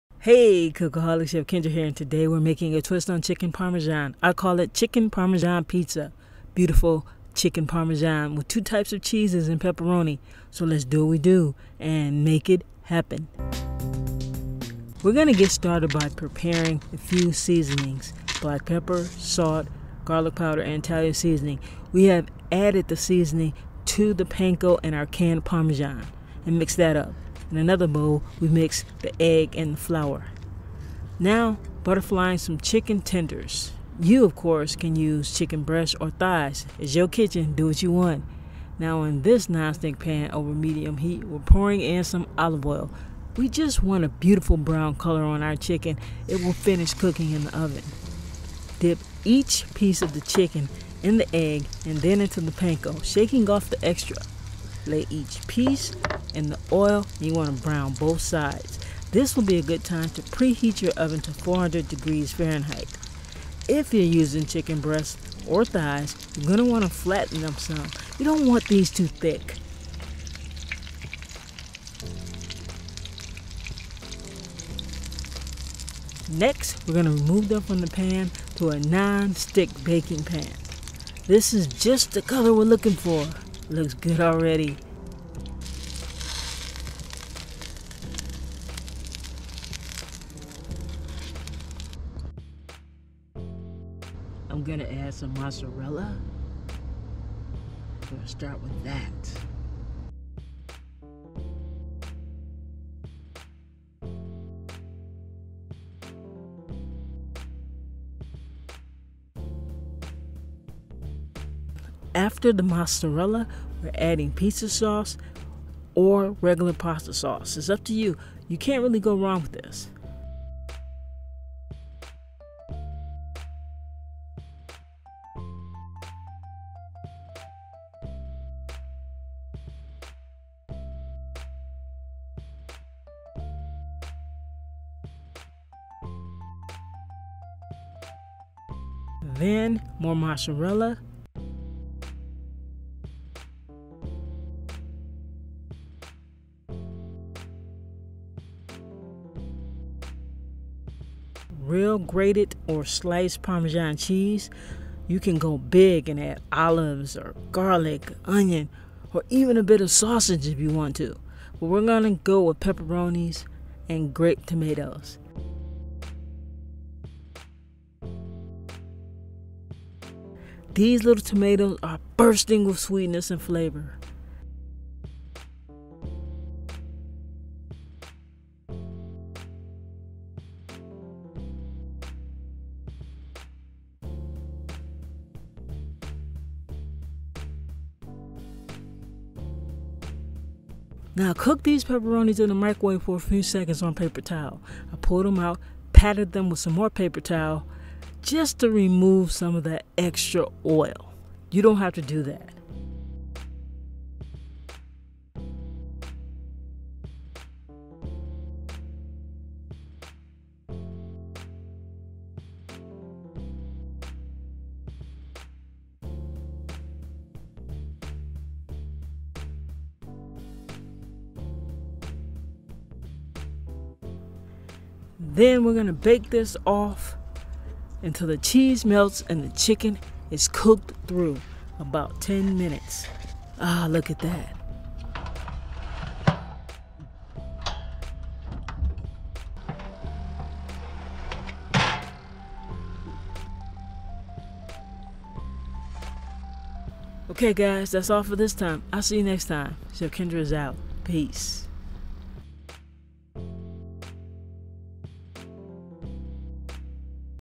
CHICKEN PARMESAN PIZZA / SO EASY / DELICIOUS
Today we're making CHICKEN PARMESAN PIZZA, so easy and delicious and a real crowd pleaser.

Chef Kendra

► CAMERA
SONY ZV-1